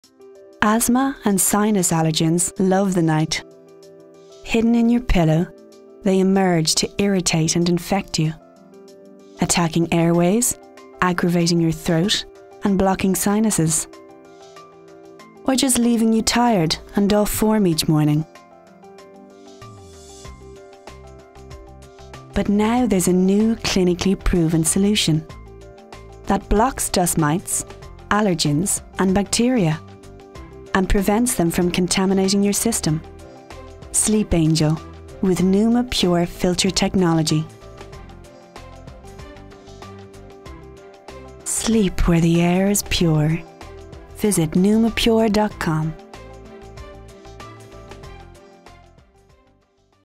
SleepAngel. The Pillow with a Filter. Information video
Why we can all benefit from sleeping in purer air and reducing our exposure to dust mites, allergens, germs and bacteria inside our pillows.
SleepAngel - The Pillow with a Filter - is scientifically proven to block these impurities, helping you to a better sleep and a Good Morning.